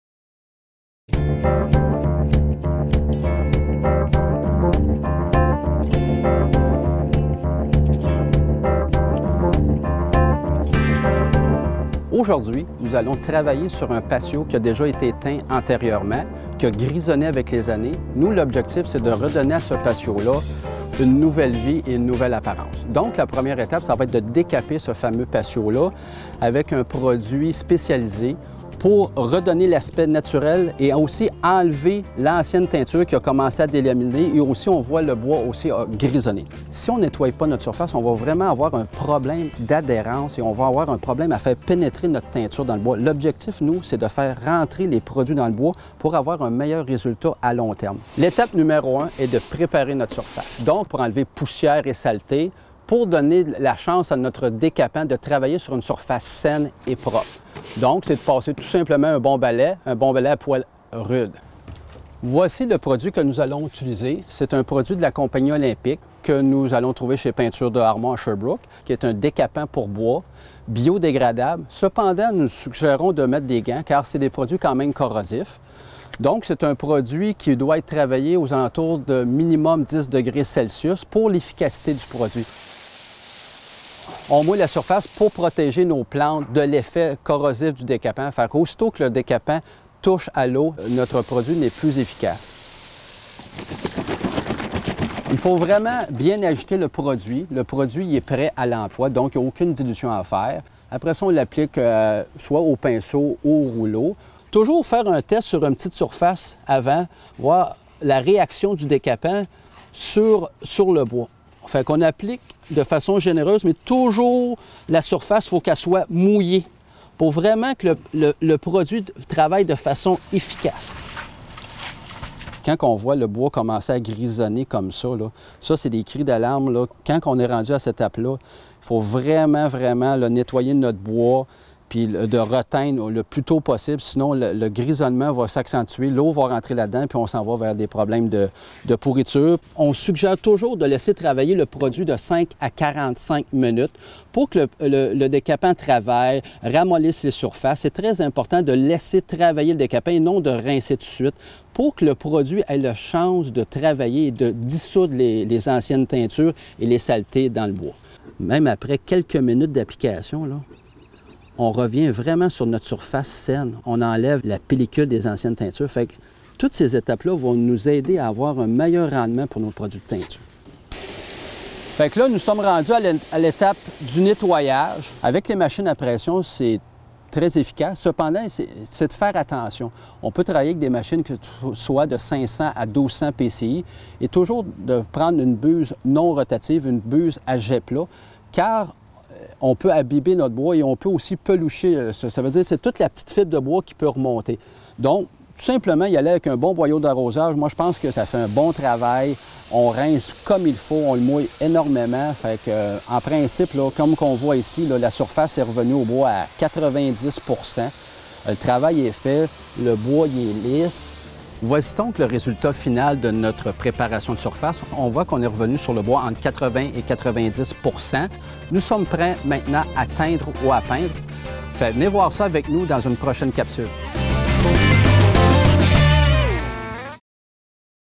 Reportage corporatif : première étape essentielle pour refaire la finition d'un patio
Avec l'usure des années, un patio extérieur peut grisonner et prendre une apparence fatiguée. Le bois grisonne à mesure que la teinture s'écaille et que la surface est exposée aux éléments.